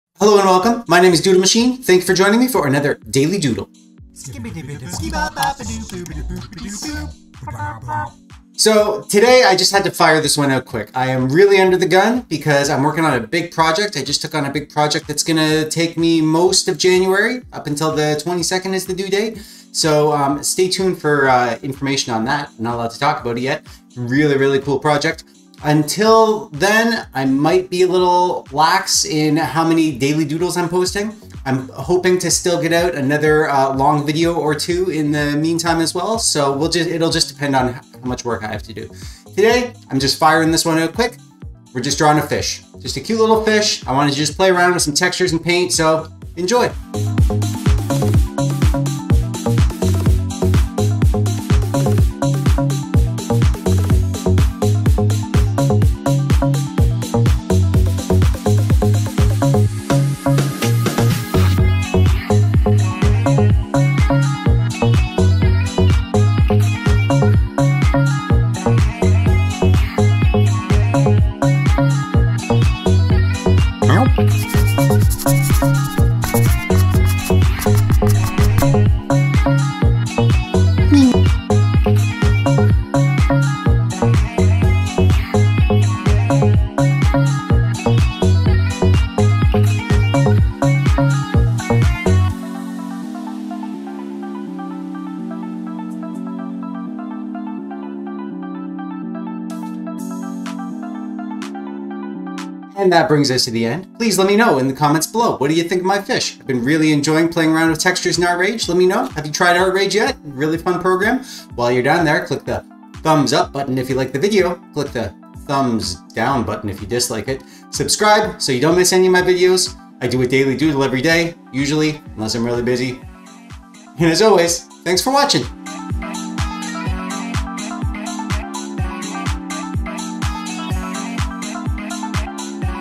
Fishy Fish ~ Artrage 6 painting
Just wanted to play around with some ArtRage textures and draw a cute little fish. That's it. Just a fish.